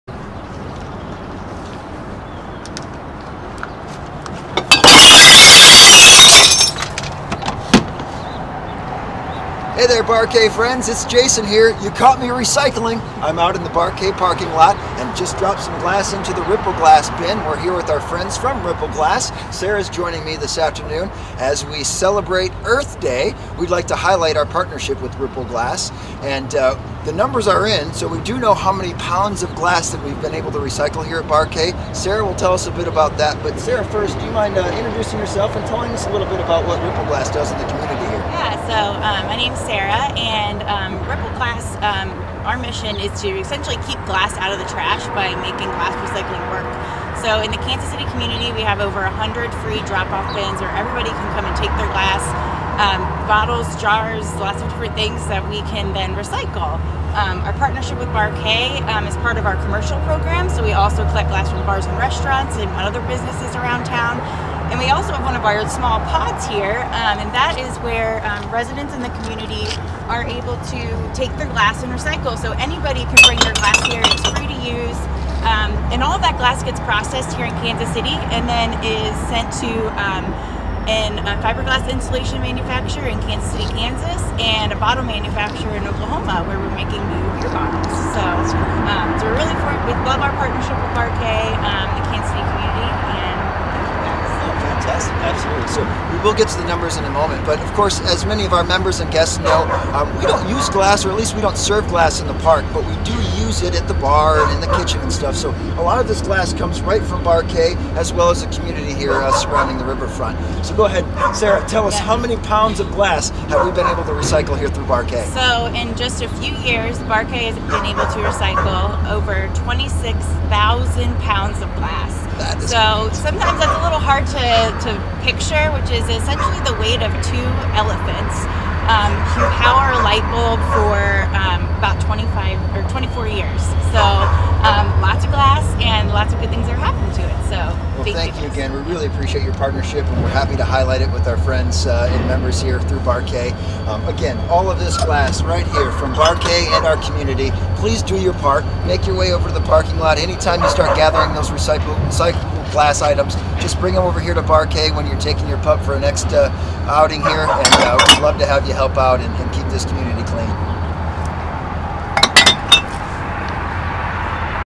Earth Day 2021 - Bar K + Ripple Glass
Did you know that glass is one of the few items that is 100% recyclable and as Americans, we dispose of over 10 million metric tons of glass every year? In celebration of earthday2021, we want to highlight an incredible community partner who helped us recycle over 26,000 lbs of glass to date! With over 100 locations in the Kansas City area, there's sure to be a Ripple Glass drop location near you!